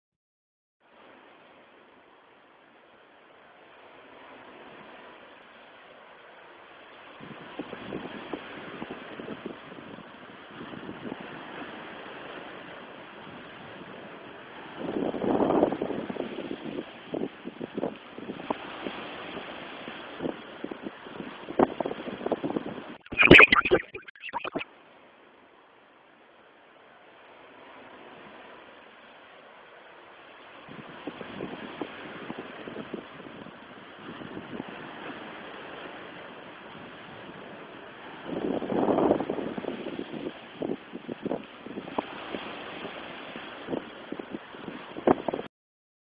Qik - Beach view from balcony room at Hotel Lido Mediterranee, Taormina by docteled
Streamed by docteled in Italy. Qik is the fastest way to upload videos to YouTube from your mobile phone.